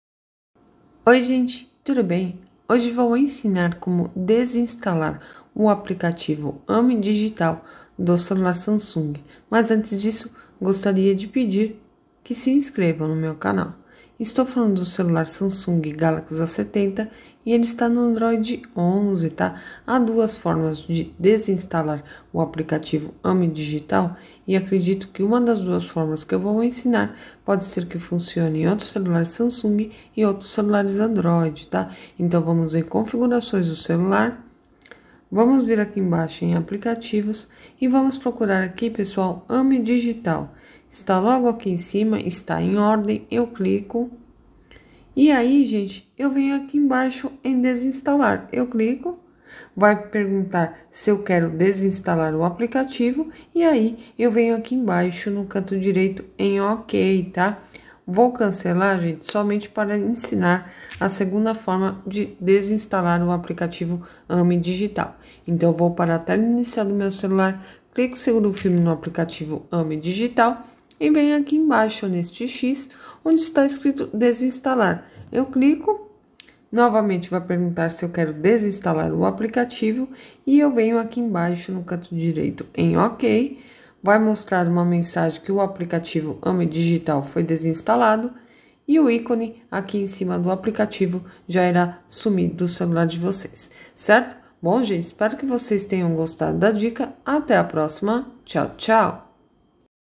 Como desinstalar o App AME DIGITAL do celular SAMSUNG
Veja neste vídeo como tirar o Aplicativo Ame Digital do celular SAMSUNG A70/ANDROID 11